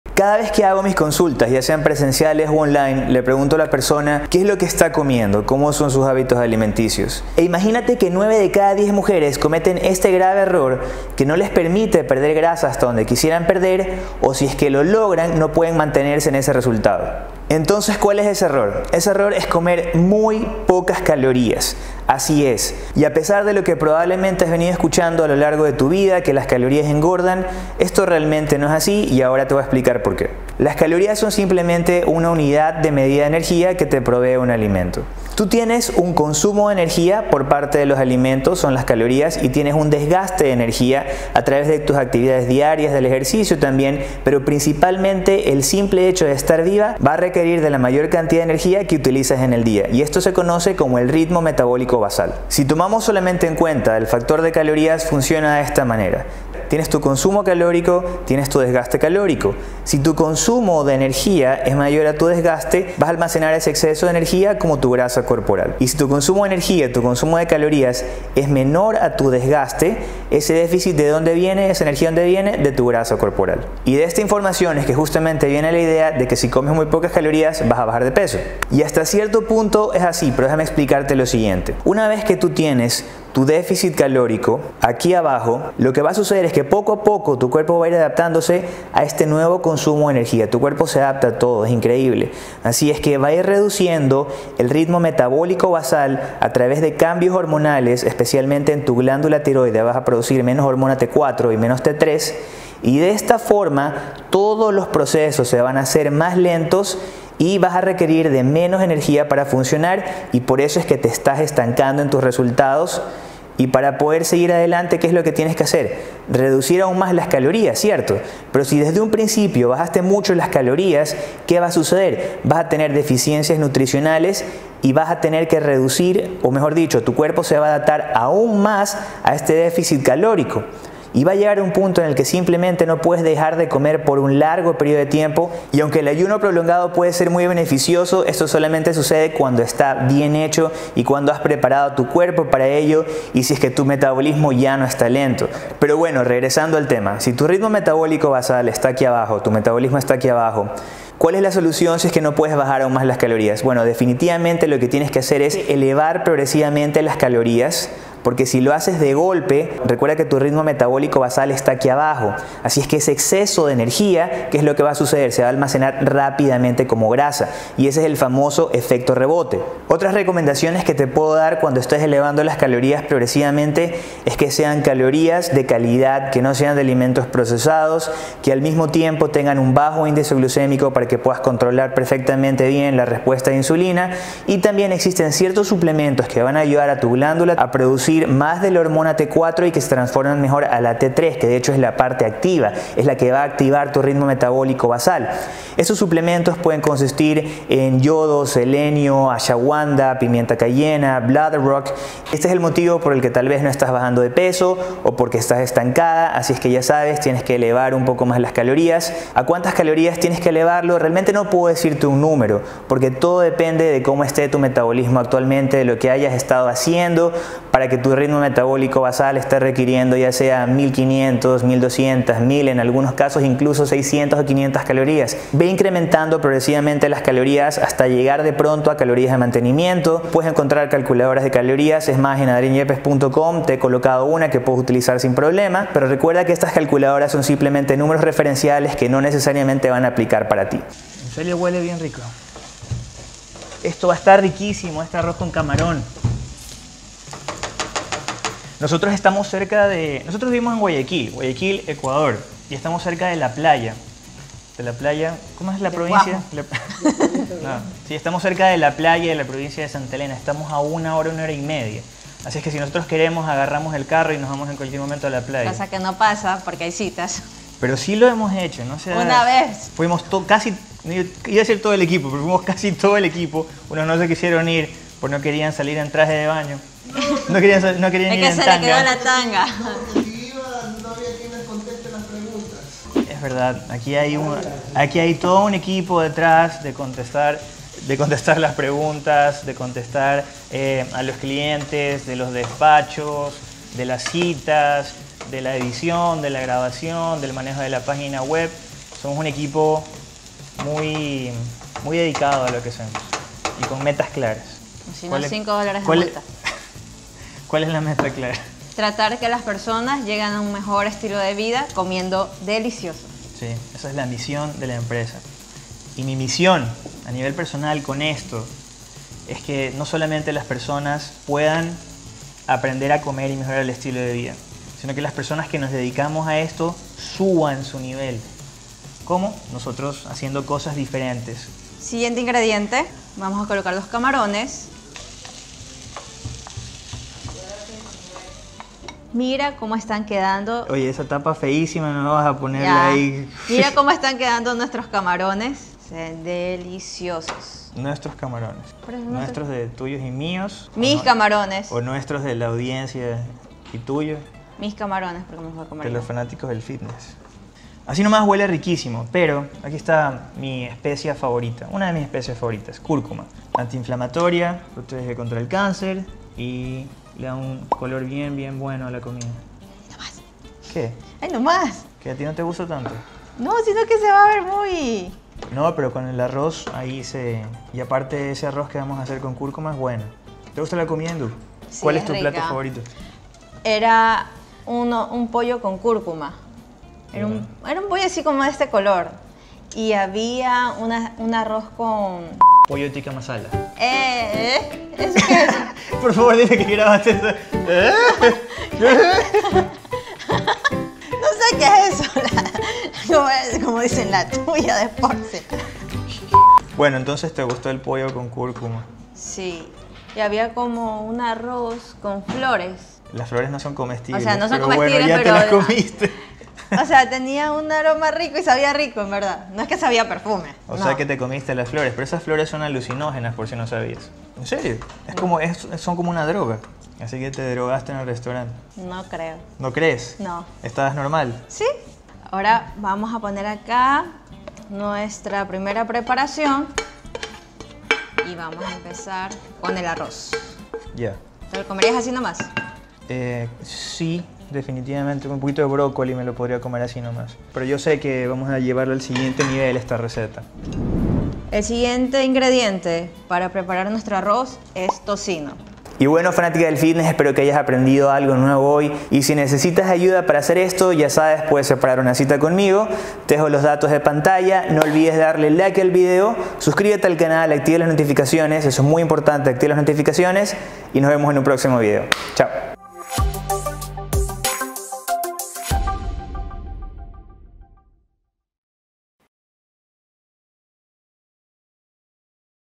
9 DE CADA 10 MUJERES Cometen Este Grave Error Cuando Quieren Perder Grasa
9 DE CADA 10 MUJERES Cometen Este Grave Error Cuando Quieren Perder Grasa

Cada vez que hago mis consultas ya sean presenciales u online, le pregunto a la persona qué es lo que está comiendo, cómo son sus hábitos alimenticios e imagínate que 9 de cada 10 mujeres cometen un grave error que no les permite perder grasa hasta donde quisieran perder, o si lo logran, no pueden mantenerse en ese resultado.¿Cuál es ese error? El error es comer muy pocas calorías. A pesar de lo que has venido escuchando a lo largo de tu vida, que las calorías engordan, esto realmente no es asi, y ahora te voy a explicar por que.Las calorías son simplemente una unidad de medida de energía que te provee un alimento. Tu tienes un consumo de energía por parte de los alimentos (las calorías) y tienes un desgaste de energía, a través de tus actividades diarias, del ejercicio también, pero principalmente el simple hecho de estar viva va a requerir de la mayorcantidad de energía que utilizas en el día, lo que se conoce como Ritmo Metabólico Basal.Si tomamos solamente en cuenta el factor de calorías, funciona de esta manera: tienes tu consumo calórico, tienes tu desgaste calórico. Si tu consumo de energía es mayor a tu desgaste vas a almacenar ese exceso de energía como tu grasa corporal. Si tu consumo de energía (calorías) es menor a tu desgaste, ese déficit viene de tu grasacorporal.De esta información justamente viene la idea de que si comes muy pocas calorías vas a bajar de peso, y hasta cierto punto es así, pero déjame explicartelo en este video